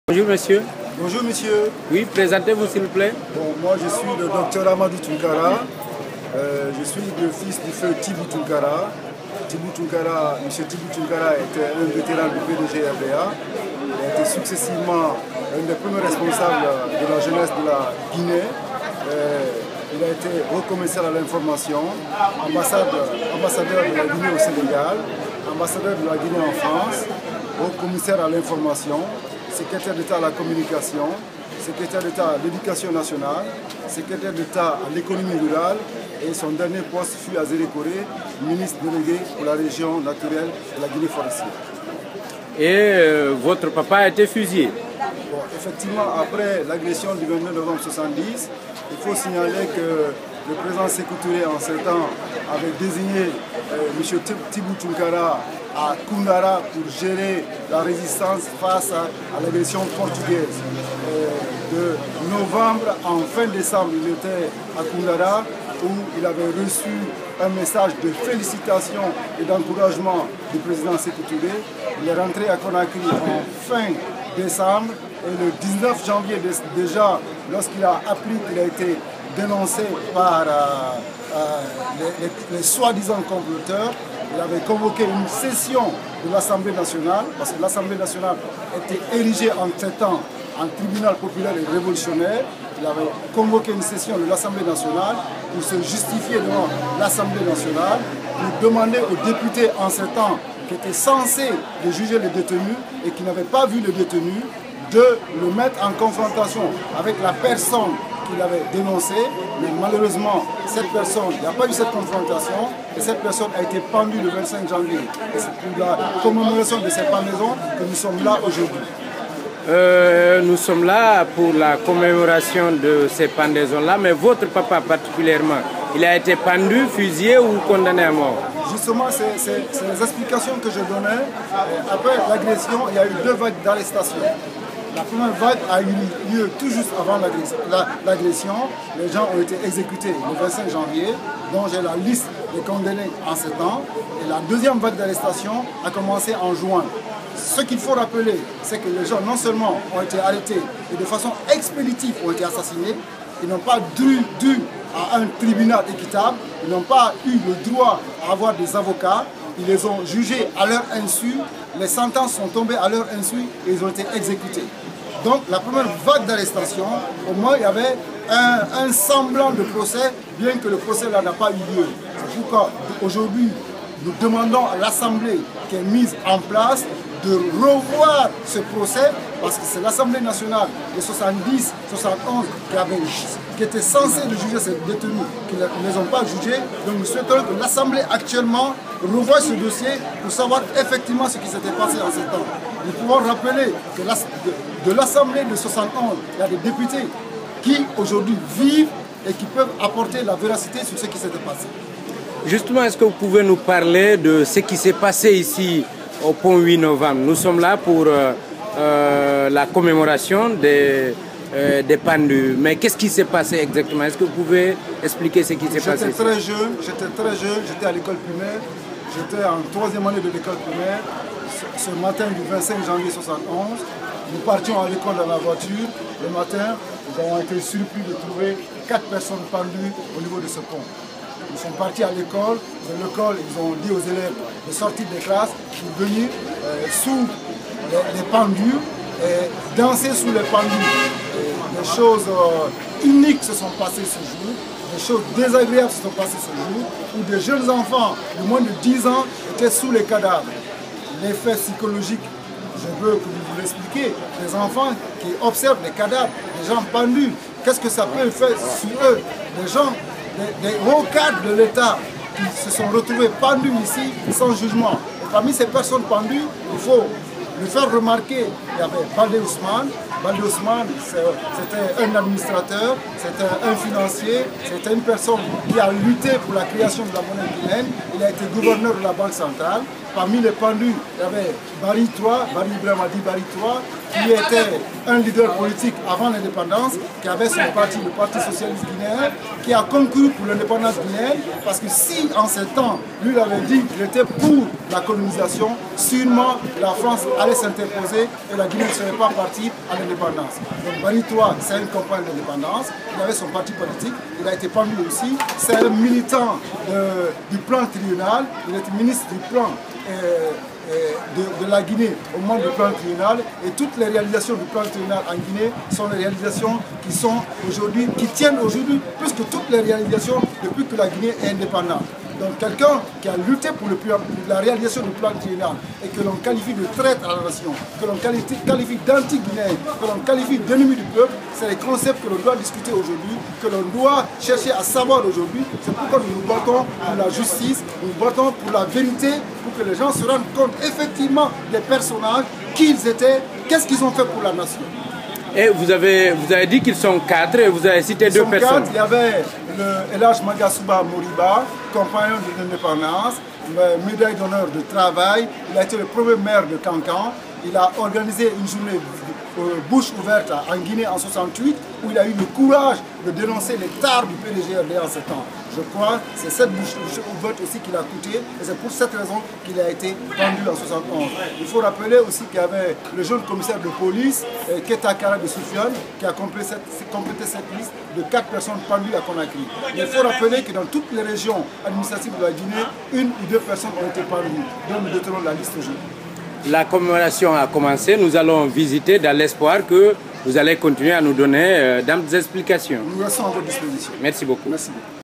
Dr. Amadou Tounkara, fils de feu Tibou Tounkara : "les gens
Dr. Amadou Tounkara, fils de feu Tibou Tounkara : "les gens ont été arrêtés t exécutés de façon expéditive".
Mon papa a été un vétéran du PDG, un des premiers responsables de la Jeunesse, ancien ambassadeur de la Guinée au Sénégal, ancien ambassadeur de la Guinée en France, ancien haut commissaire à l'Information, ancien secrétaire d'Etat à la Communication, ancien secrétaire d'Etat à l'Education, ancien secrétaire d'Etat à l'Economie rurale, ancien ministre délégué pour la région naturelle de la Guinée Forestière.
Les gens ont été arrêtés et exécutés de façon expéditive par l'Assemblée nationale populaire de 1971...
Nous demandons à l'Assemblée nationale actuelle de revoir ce procès de 1971. Je rappelle qu'il y a des anciens députés de 1971 qui sont encore vivants et qui peuvent témoigner...